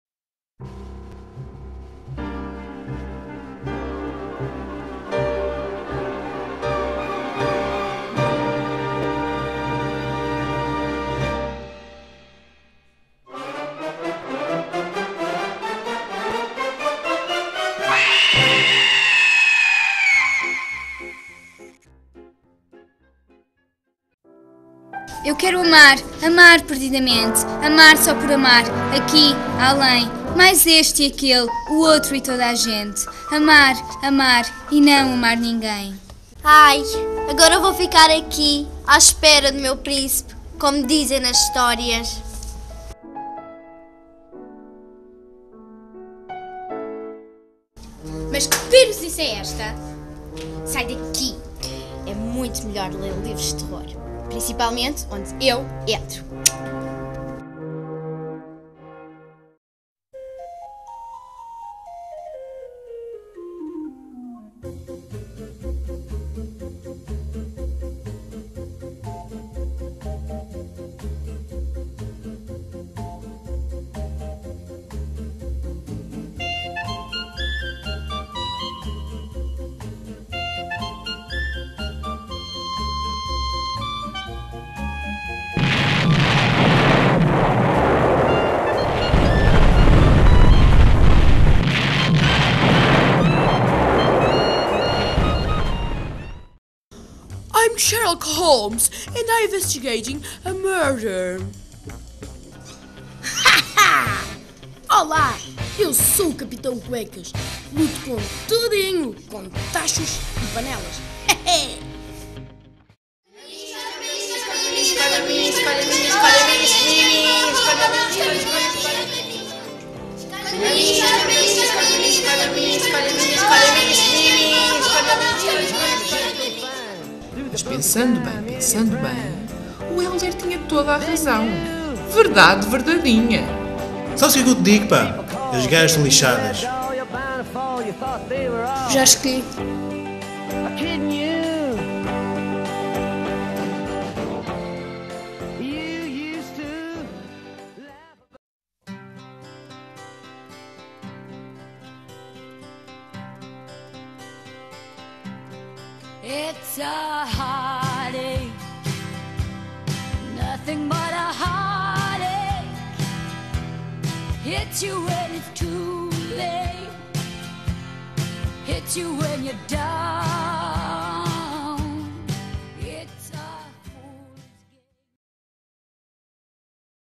A Escolha - Biblioflmes
Participação no concurso Bibliofilmes para promoção do  livro e da leitura.

Agrupamento de Escolas da Venda do Pinheiro
Centro de Recursos Poeta José Fanha